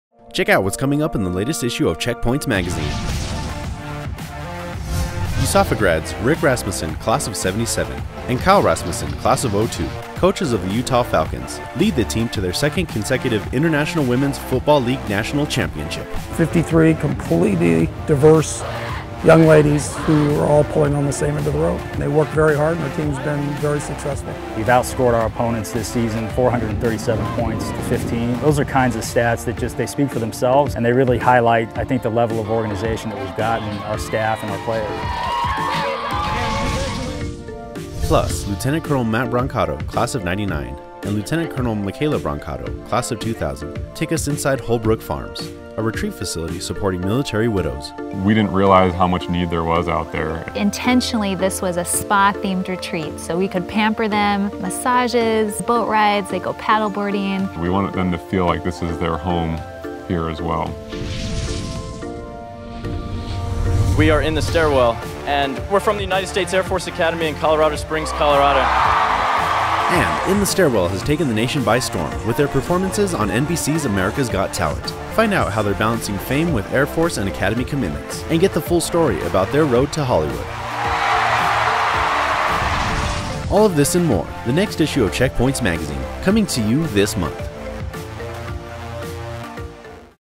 Checkpoints - September 2017 Preview
Check out what's coming up in the September 2017 issue of Checkpoints magazine!